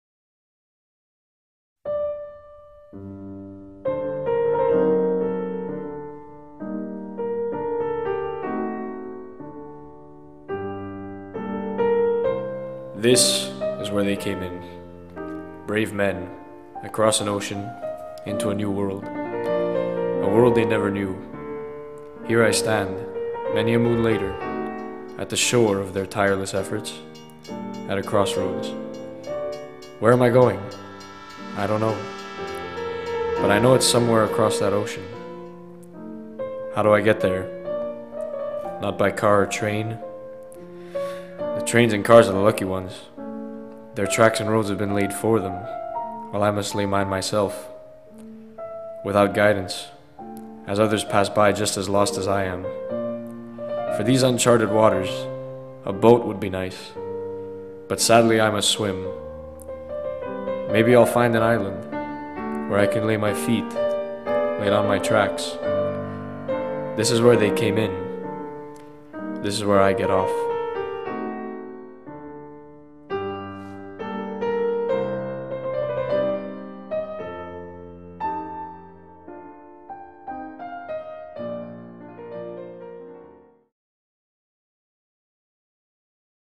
train of thought- Johnny Pisaturo
a short film about not knowing where you're going.
cinematography by riccardo marrone
written/narrated/acting by johnny pisaturo